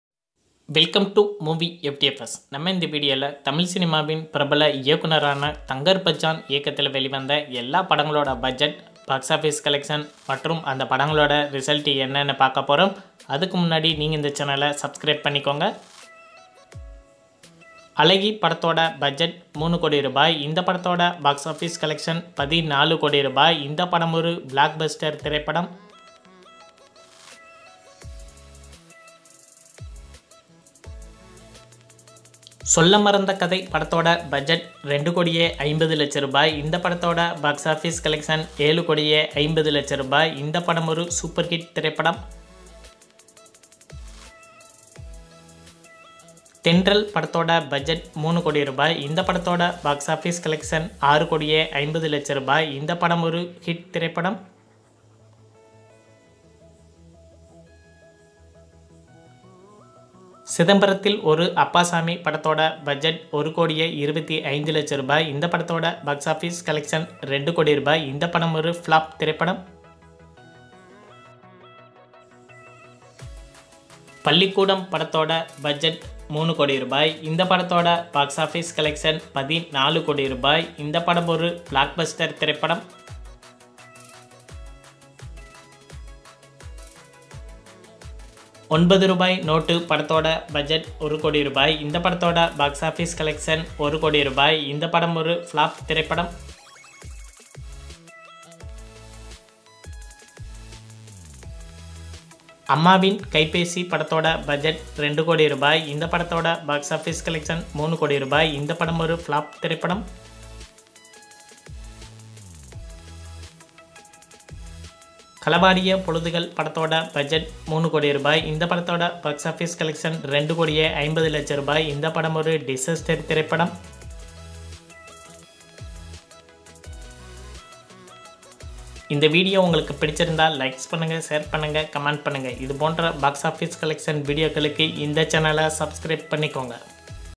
இயக்குனர் நடிகர் தங்கர் பச்சான் பட வசூல் |Director Actor Thangar Bachan Movies Box office Collection
Checkout  Movie Fdfs The Tamil Movie Director  Actor Thangar Batchan All Movies Box Office Collection Analysis With Budget and Verdict Hit Or Flop List